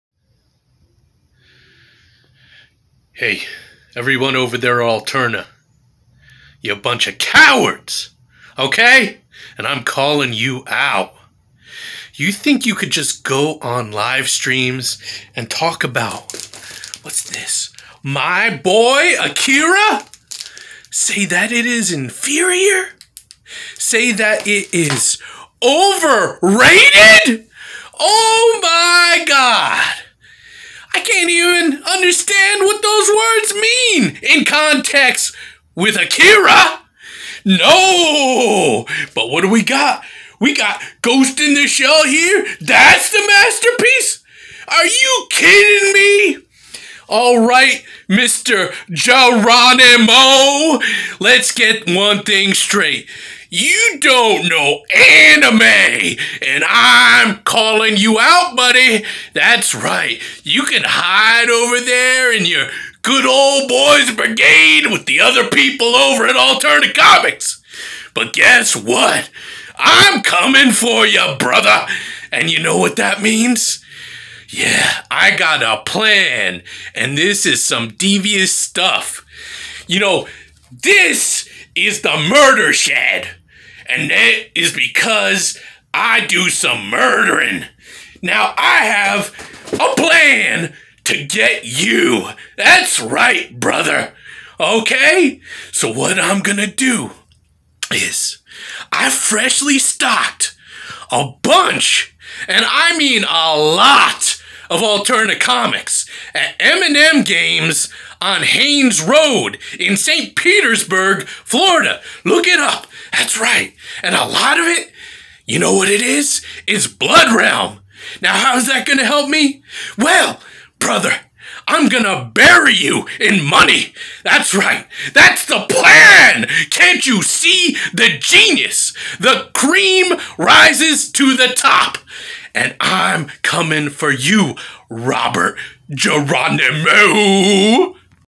CALLING OUT ALTERNA COMICS AND ROBERT GERONIMO PROMO
Wrestling promo video calling out Alterna Comics and Robert Geronimo.